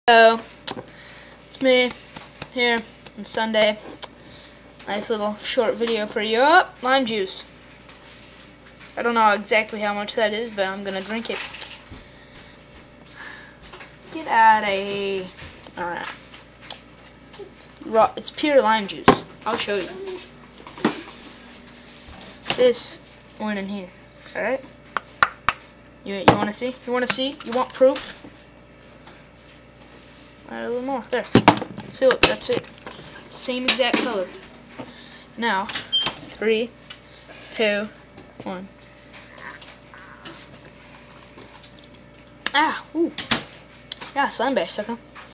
One Take Sunday #1 - Shot of Lime Juice
WHAT'S UP, GUYS? watch me drink some lime juice!
I'm gonna start doing this "One Take Sunday" thing like the SHAYTARDS.

1. Funny
2. 1080p HD videos
4. I read all comments
5. IT'S FREE!

________________________________________­____.com